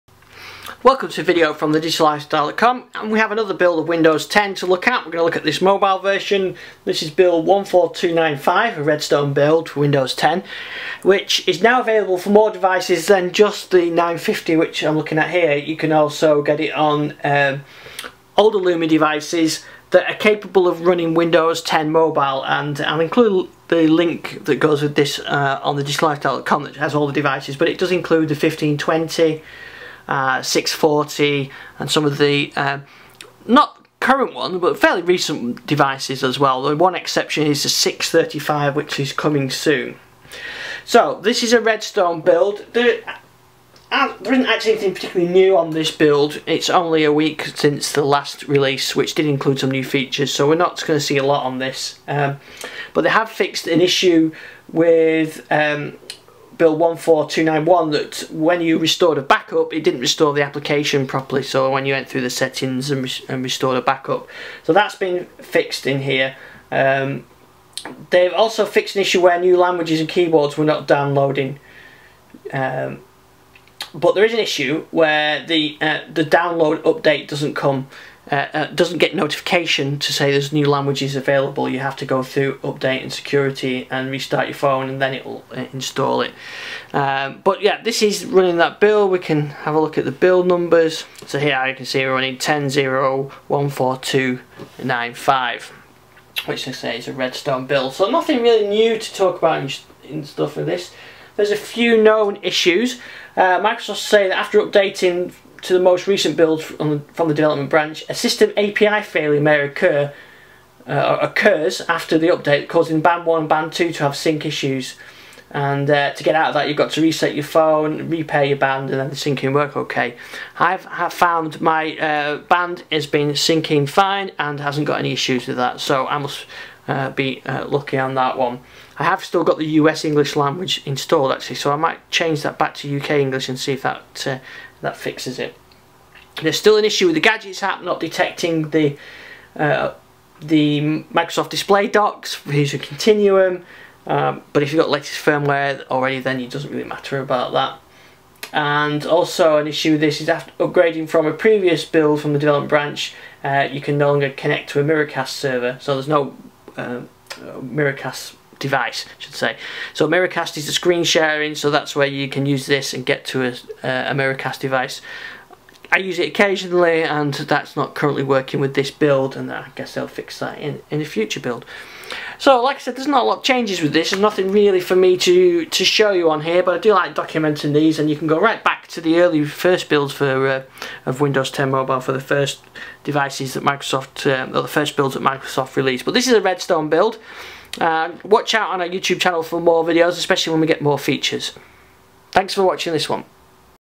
Hands on with Windows 10 Mobile Redstone build 14295
We have another build of Windows 10 Mobile to play with! This video is a hands on with another Redstone release this time its build number 14295. There are no features in this release, there a just a few fixes and known issues. I suspect we will seem new features at Microsoft Build event next week.

In this video I walk though some of the changes and know issues.